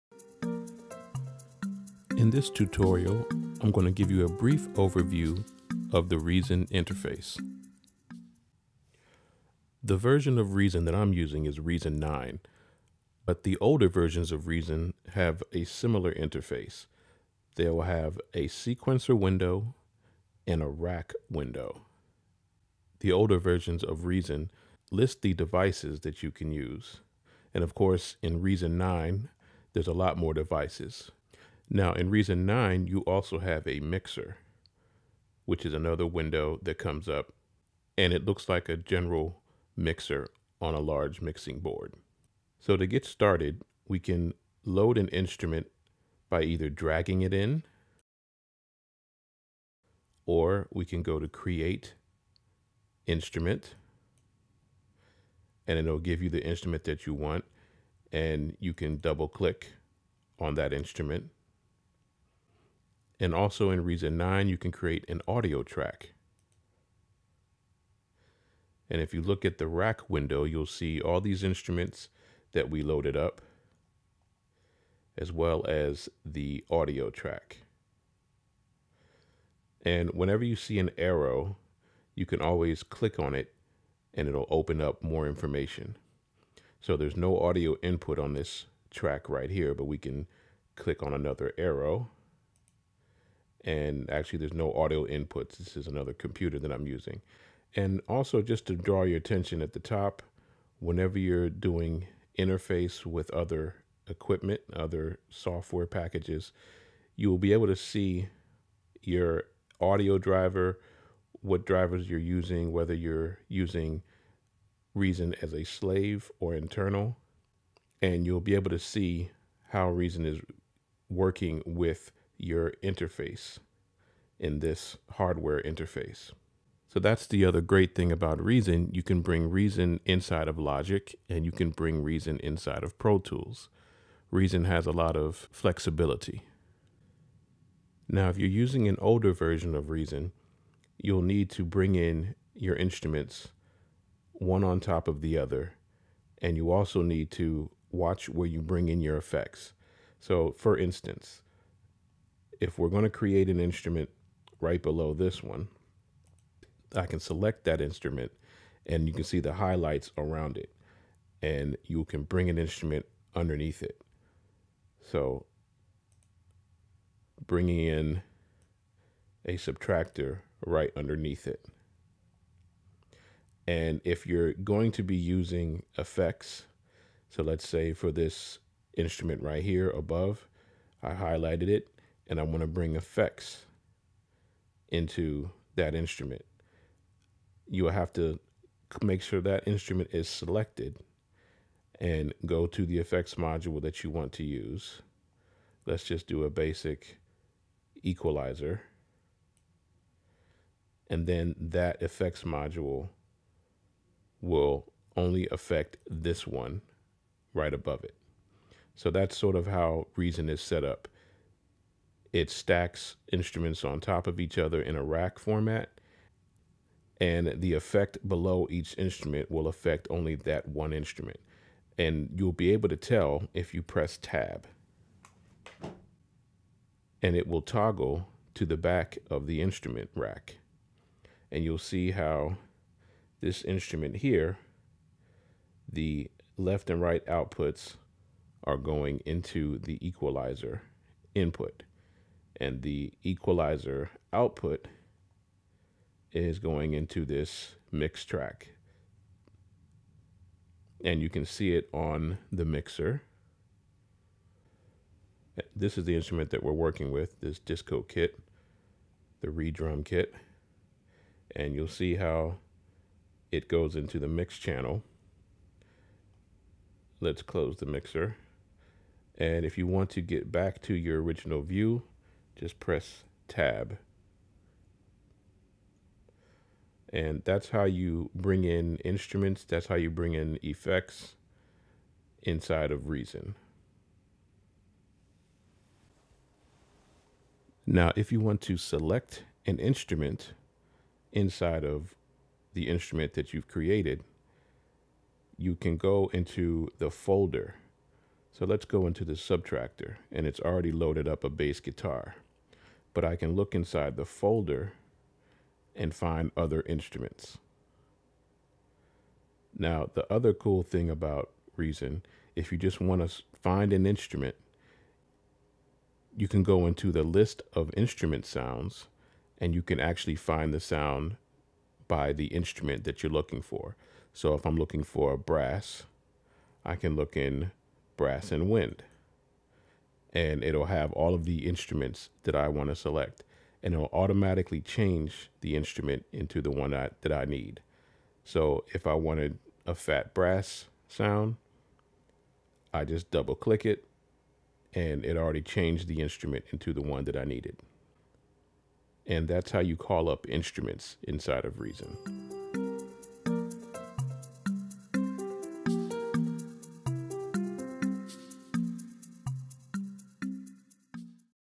Getting Started in Reason Music Software
In this tutorial you will get a basic overview of how to bring up instruments and effects in Propellerhead's Reason.